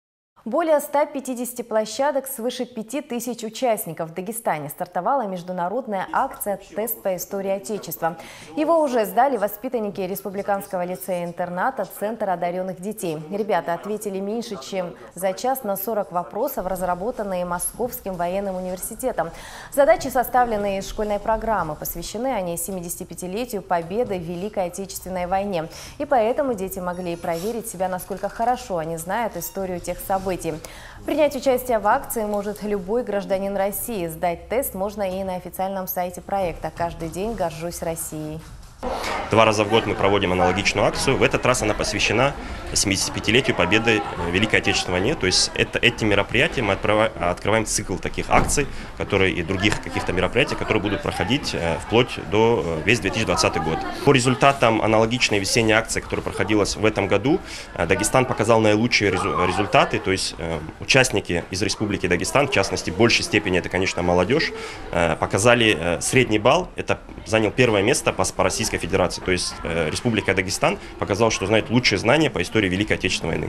В Дагестане стартовала международная акция Тест по Истории Отечества
Более 150 площадок свыше 5000 тысяч участников.  В Дагестане стартовала международная акция  Тест по Истории Отечества.  Его уже сдали воспитанники Республиканского лицея-интерната «Центр одаренных детей».  Ребята ответили меньше чем за час на 40 вопросов разработанные московским военным  университетом.